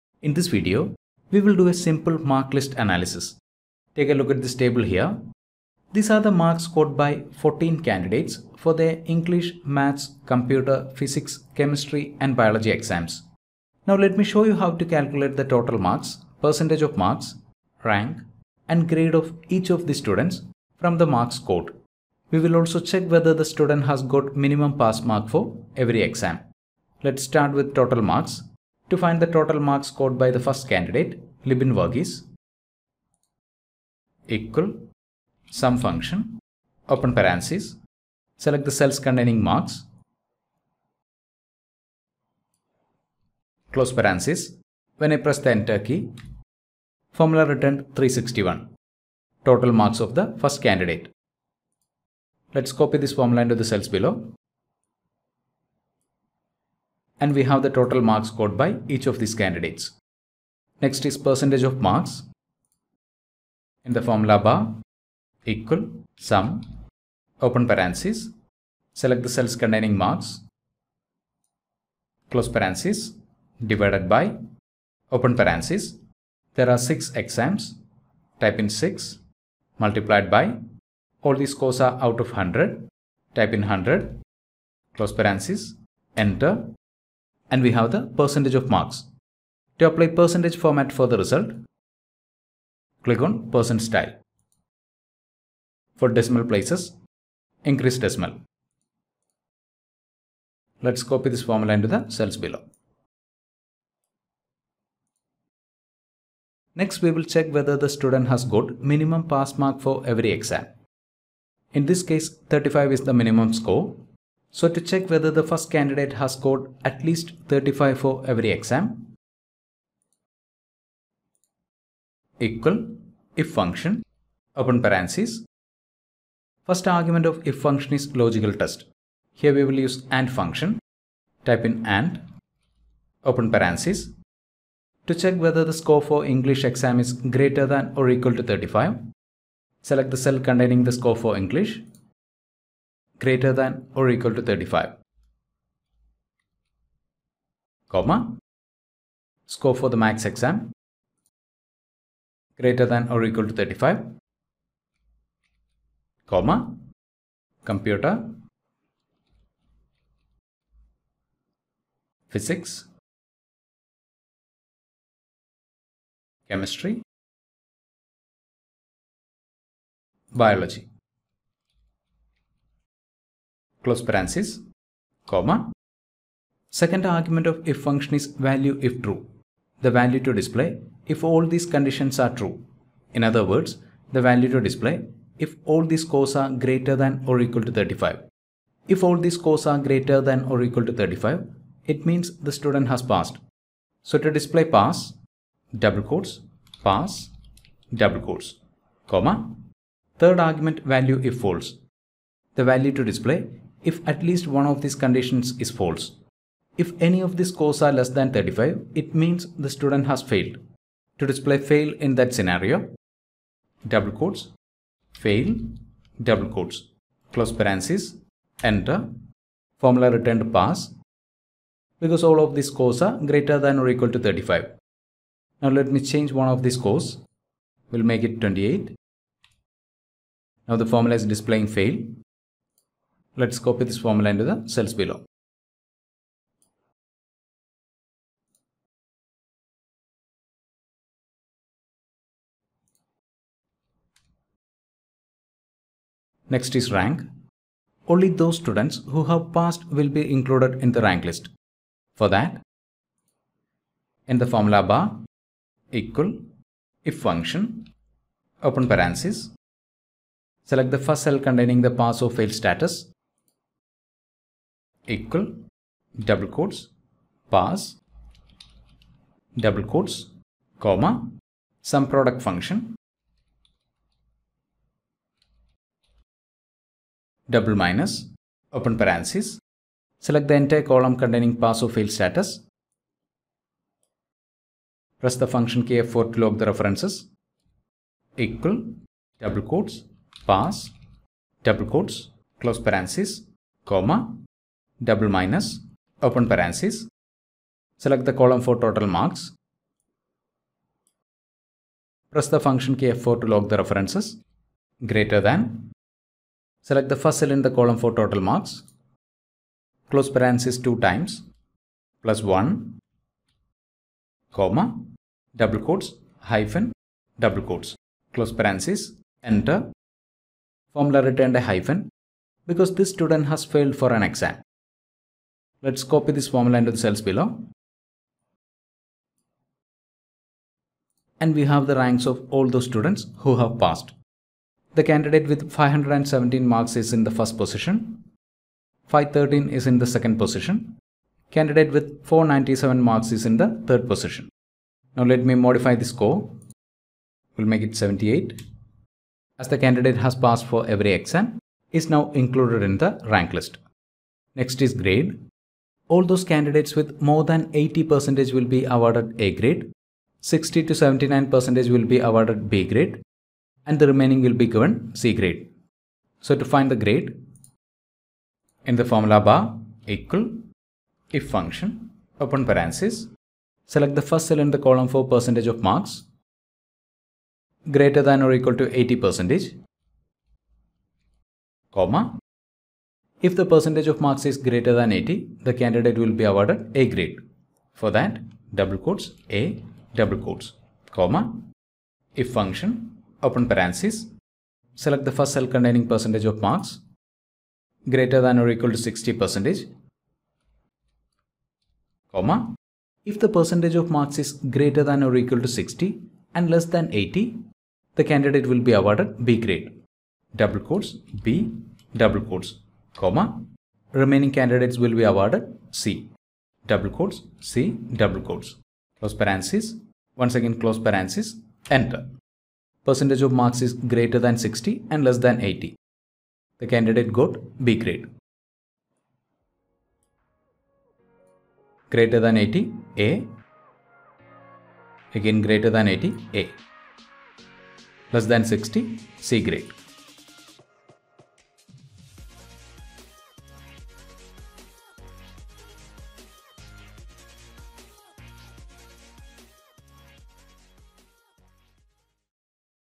Excel Formula to calculate Rank and Percentage of Marks
How to find the Total Marks, Percentage of Marks, Rank, and Grade using Microsoft Excel is explained in this video.

📊 Think Like an Analyst: Books That Helped Me
📚 Mind-Opening Reads You Can’t Miss